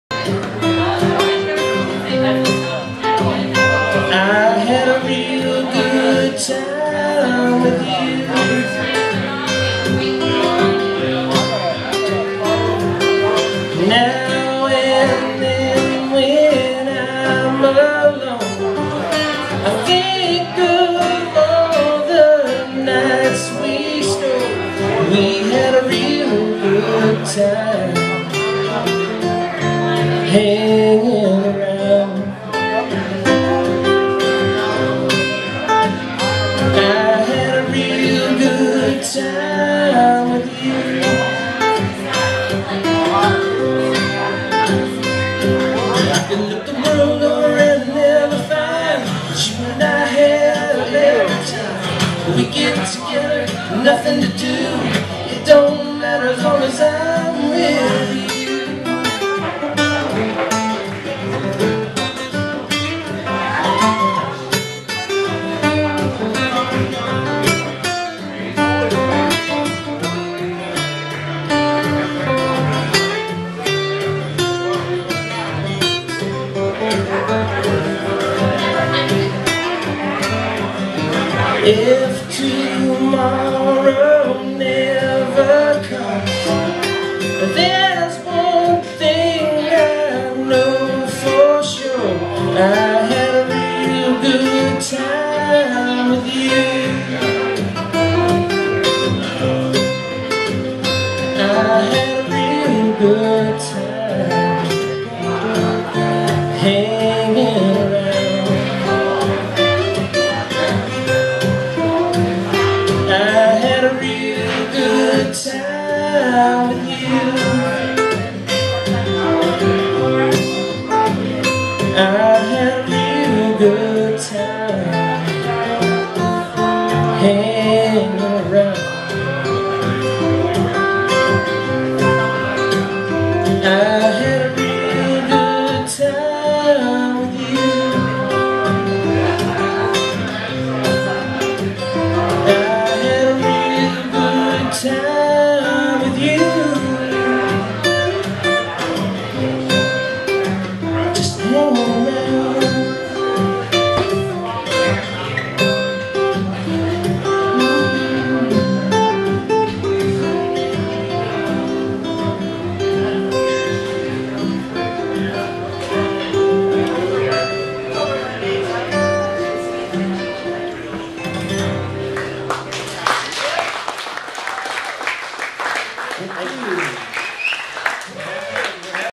Billy Russell and Chuck Seaton
Billy Russell and Chuck seaton playing an acoustic show at the Prime Cut in Bakersfield California.